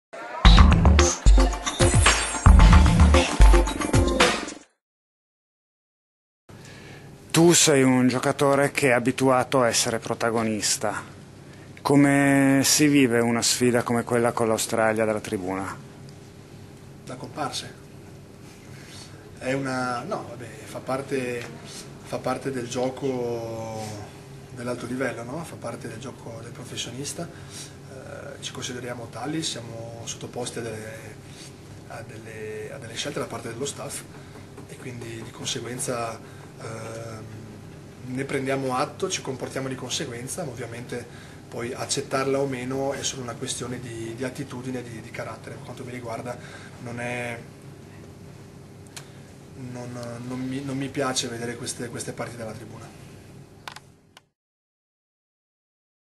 La rabbia di Mauro
La rabbia di non essere protagonista c'è e si vede. E Mauro Bergamasco fatica anche a nasconderla. Vuole giocare, si sente pronto, sta bene e accetta a malincuore le scelte dello staff tecnico. Vedere Italia-Australia dalla tribuna non gli è piaciuto, come dice qui sotto a Rugby 1823.